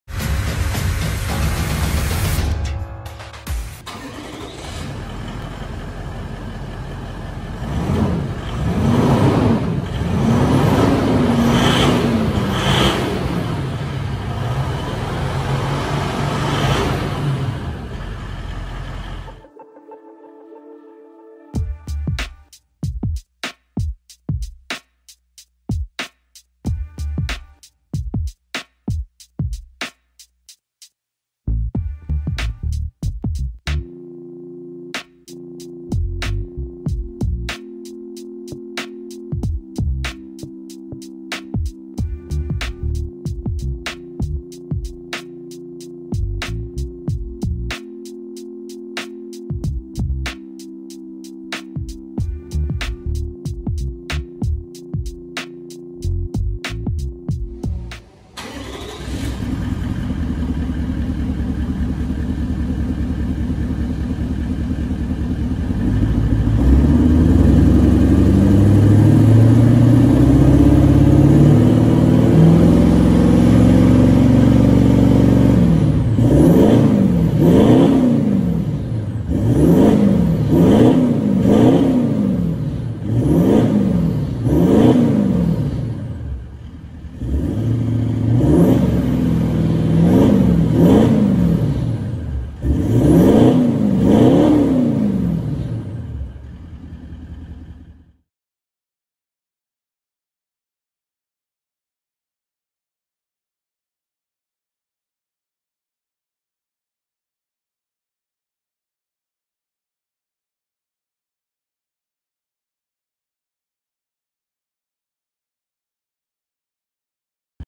200 Series Landcruiser Twin 3" Into 4" Torqit Exhaust (Louder) Before And After
200 Series Landcruiser Twin 3" Into 4" Torqit Exhaust (Louder) Before And After
This is the louder twin system that Torqit supplies for the 200 series (LC200)
Landcruiser.
They do offer another version for the pre "DPF" model 200 series that has the muffler which i also decided to purchase for when you're on the longer trips towing just to quieten it down.
This system is completely legal as it still maintains the catalytic converters they're just a high flow version. Also the system is below the legal DBs limit.